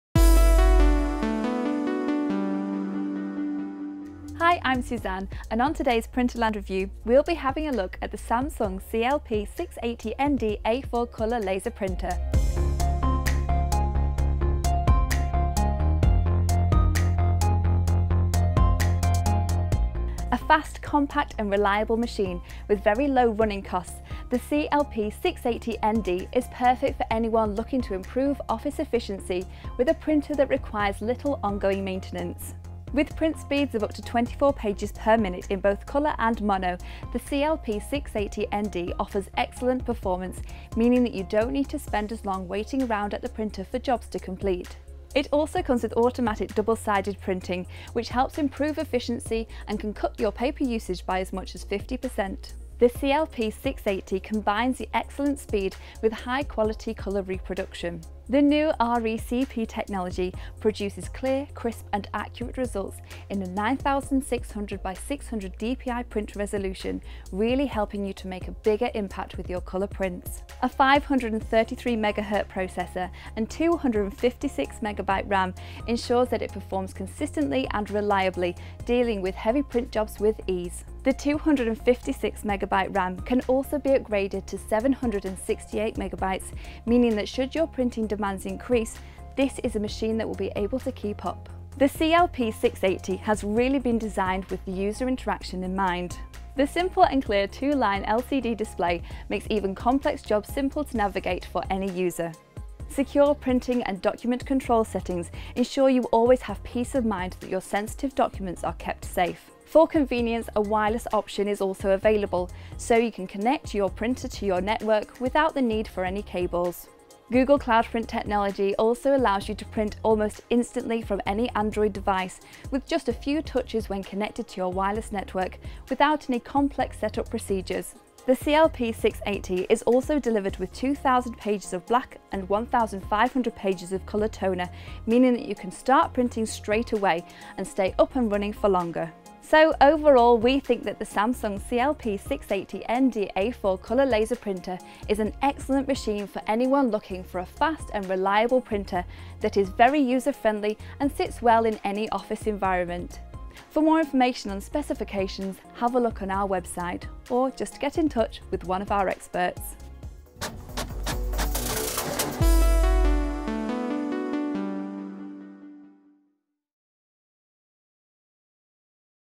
Samsung CLP-680ND A4 Colour Laser Printer
Features:

·         Up to 24ppm print in colour and mono

·         Ships with 2000 pages of black and 1,500 pages of colour toners

·         Automatic double-sided printing as standard

·         Wireless option available

·         Usb & network

·         9600 x 600 dpi

·         700MHz processor

·         256MB Ram

·         250 sheet input tray

·         50 sheet multipurpose tray for printing on wide range of media sizes and types

·         Add an additional paper tray to expand paper capacity up to 820 sheets

·         Google cloud print ready for printing from mobile devices

·         Simple and clear 2 line LCD display with secure printing and document control via the numeric keyboard